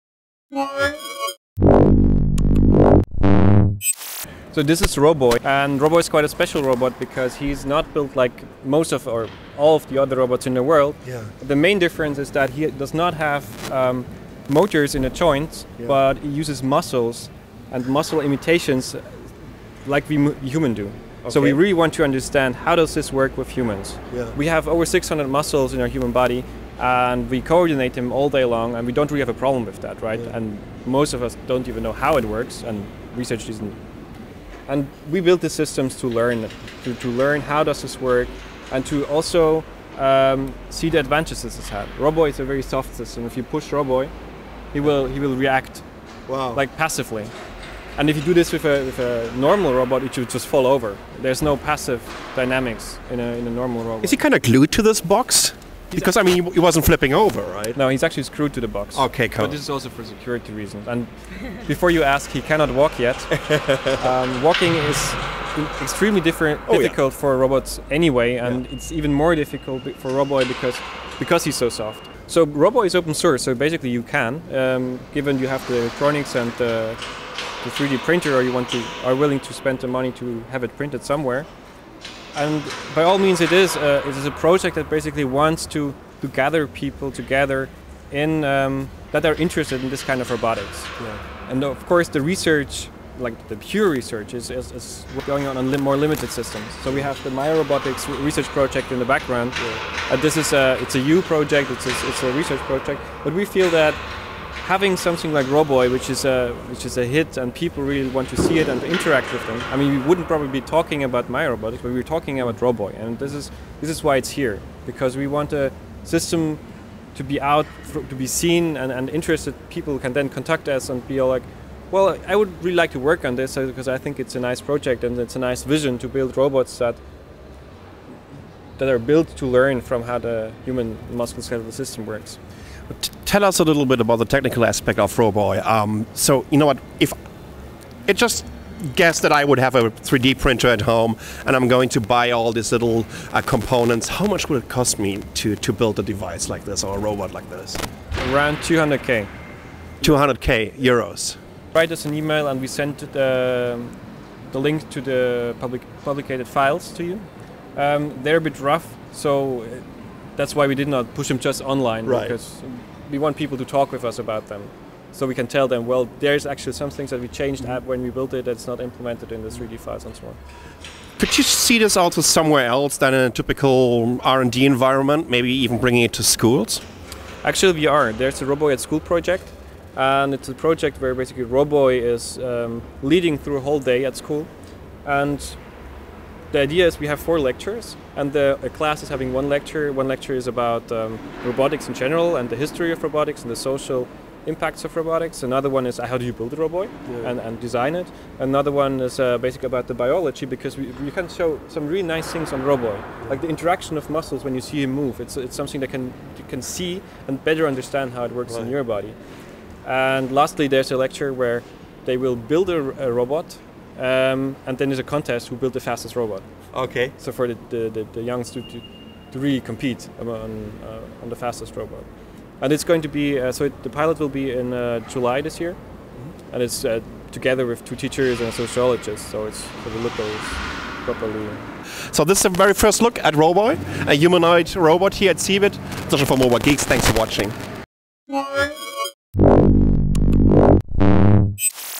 Roboy - An Advanced Humanoid Robot
Roboy Robot Roboy is a humanoid Robot that came to being through crowd funding! He is built to replicate the human form factor he doesn't have motors as muscles but springs. One of Roboy's creators gives us the run down on this robot.